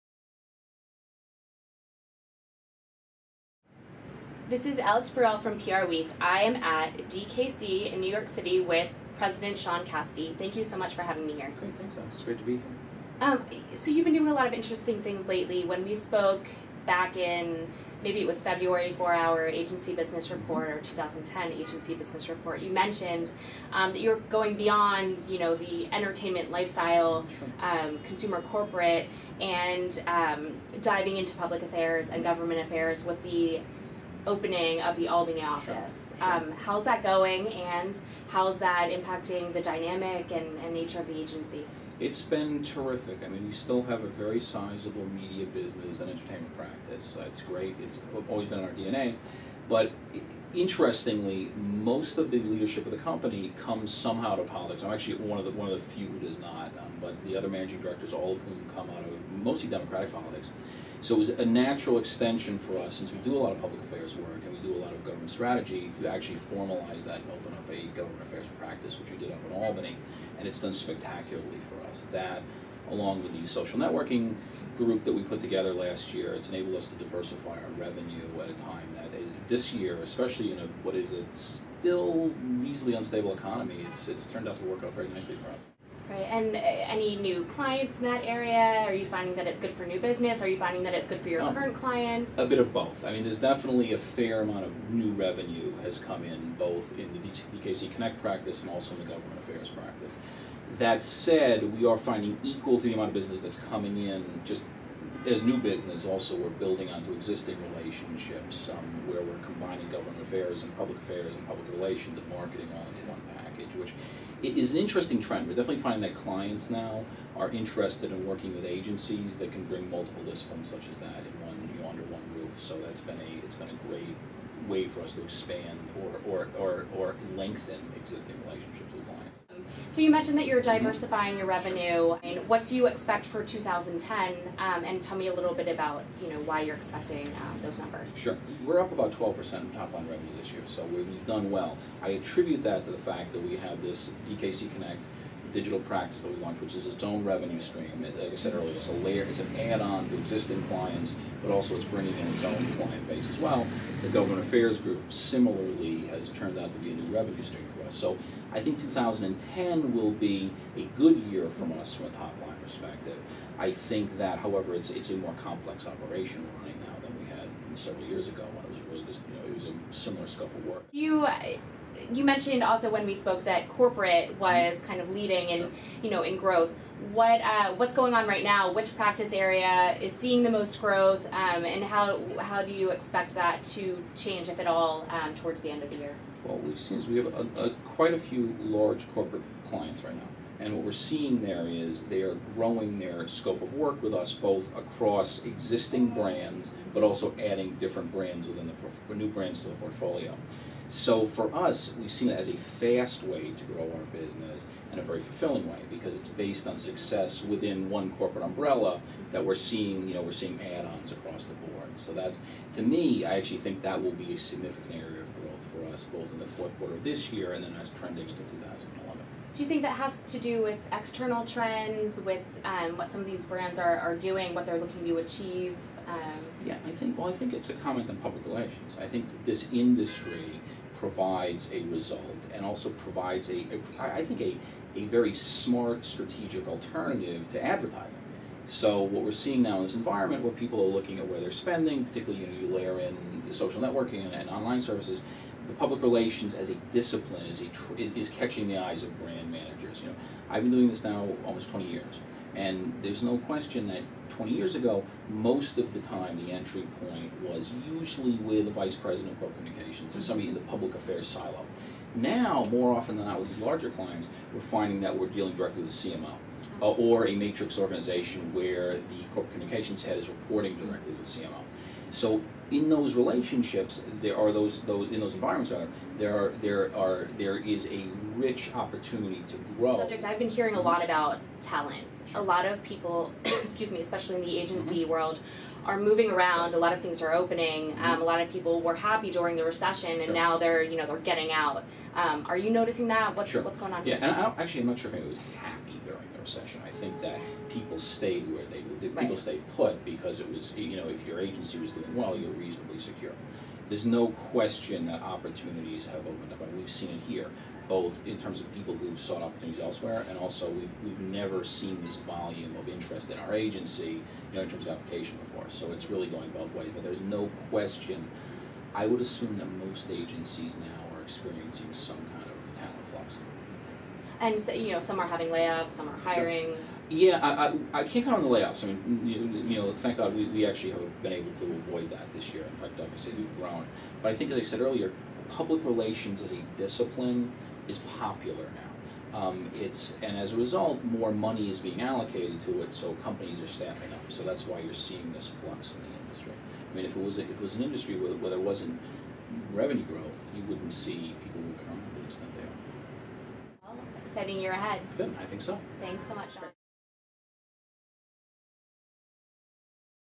Video: Sean Cassidy, president, DKC
Alexandra Bruell talks to DKC president Sean Cassidy about the agency's new public affairs practice in Albany and what he hopes to achieve for 2010.